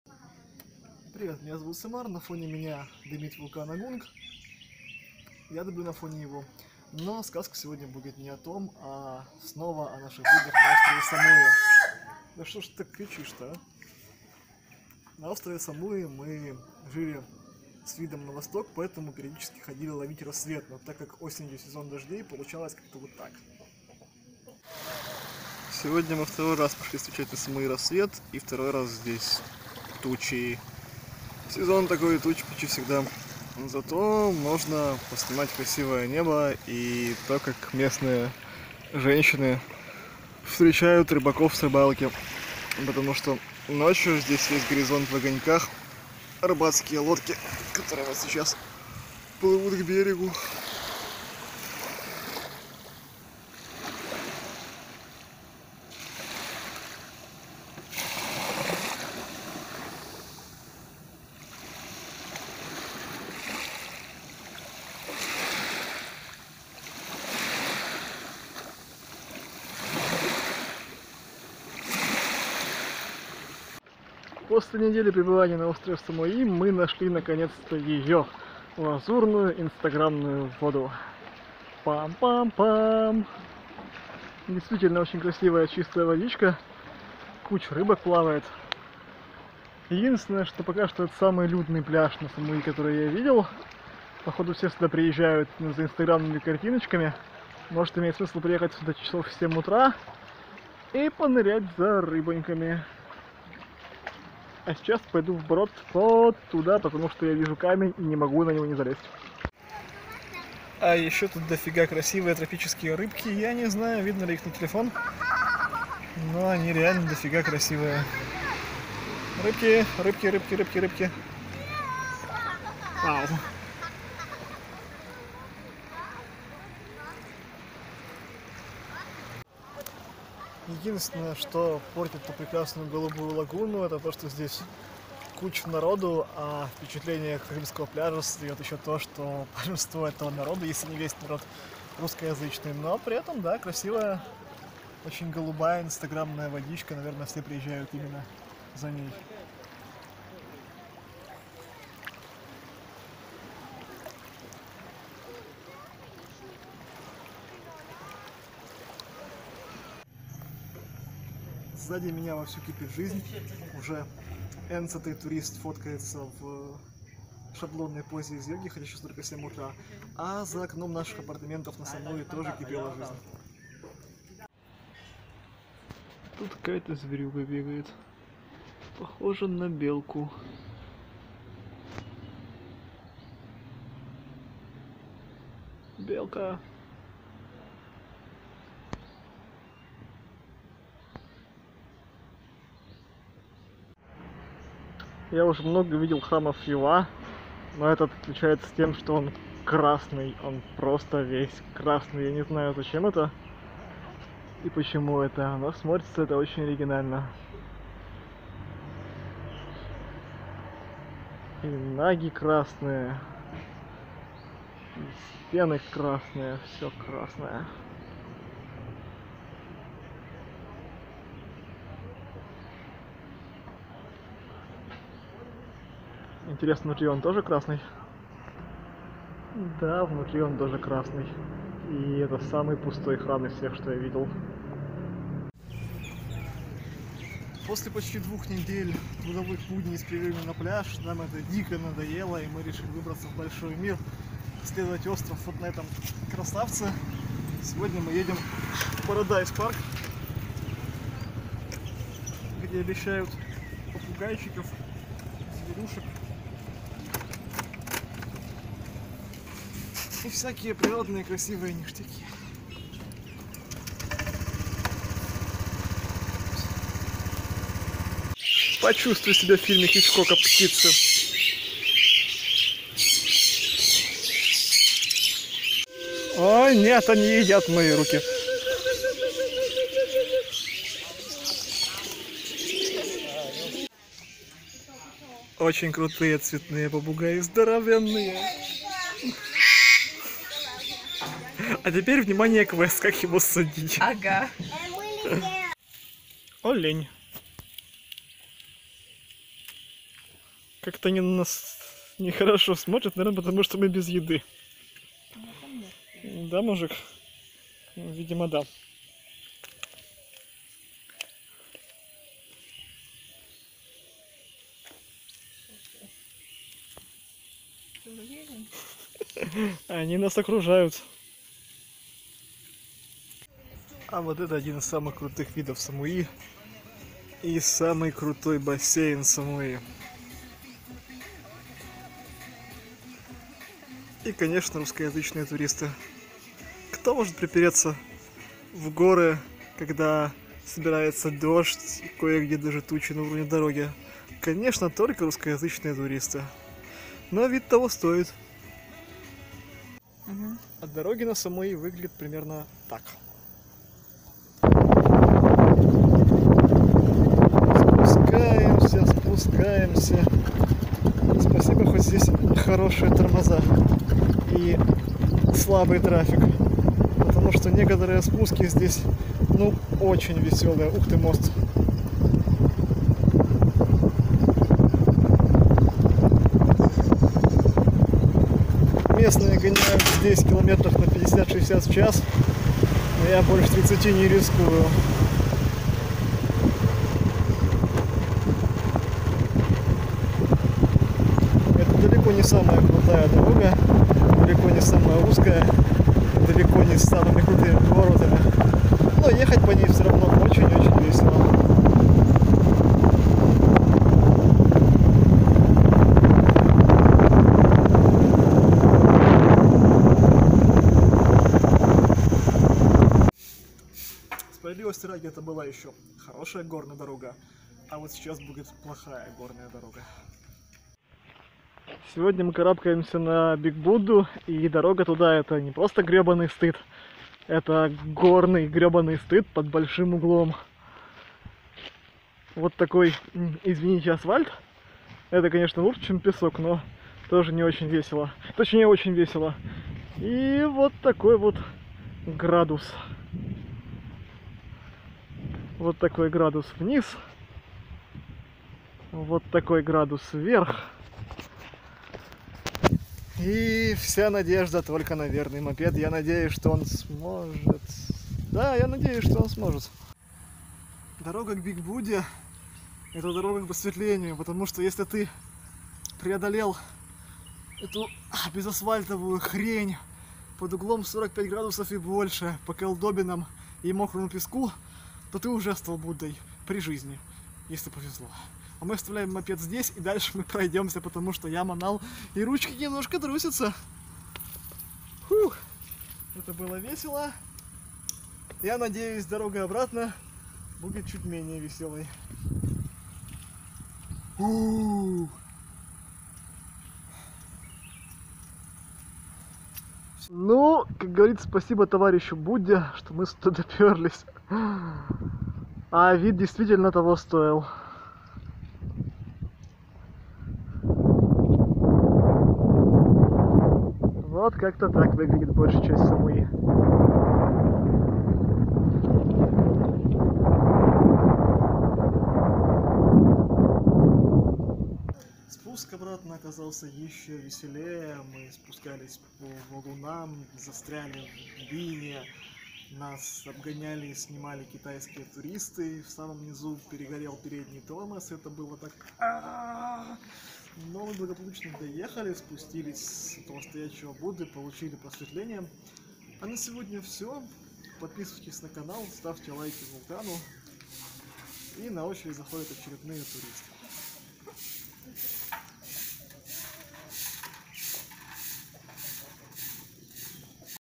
Кругосветка. Часть 31: Безумный Будда, дорога к просветлению
Ледянящее душу видео о том, как мои руки едят птицы, как мы премся по горным дорогам на самую высокую точку Самуи и как мы встаем в 5 утра, чтобы увидеть рассвет.